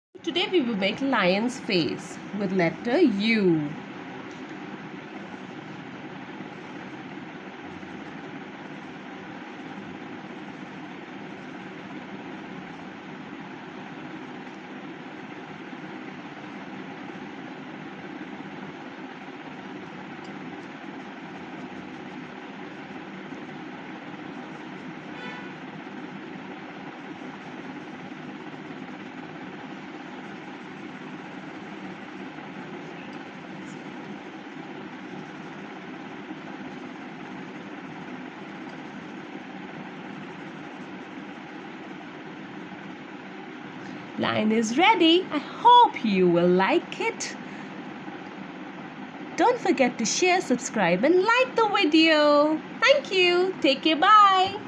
This superb trick will help in drawing lion's face easily, a pencil sketch
how to draw lion's face , a pencil sketch Draw lion face ,how to draw lion face,how to draw lion face step by step,pencil sketch drawing,Pencil sketch,pencil sketching